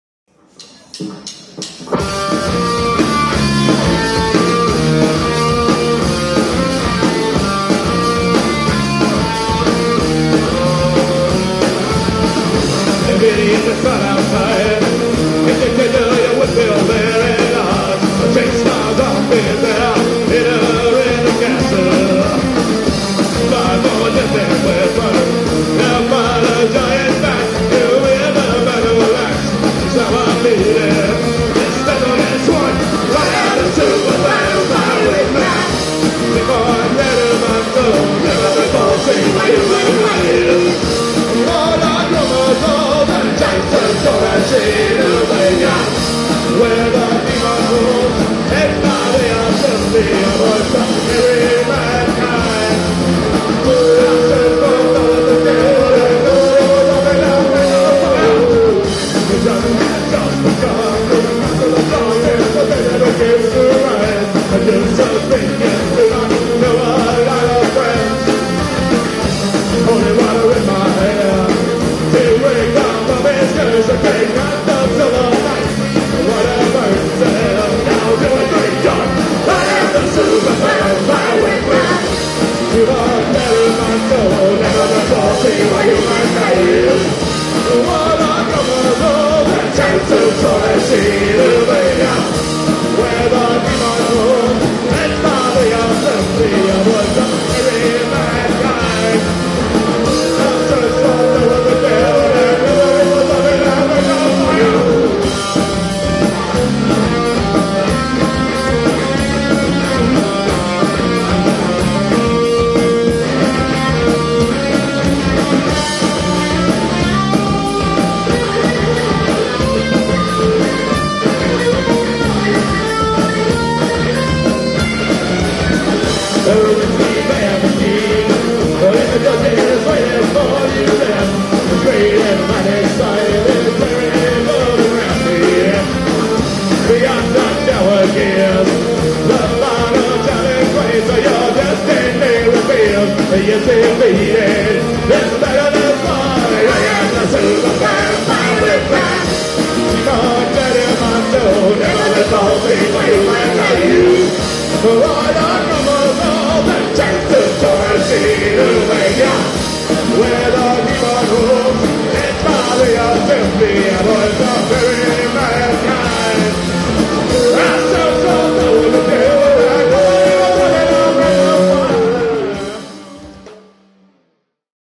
The Millipede @ Red 7 Club
The Millipede at the Red 7 Club in Austin, Texas - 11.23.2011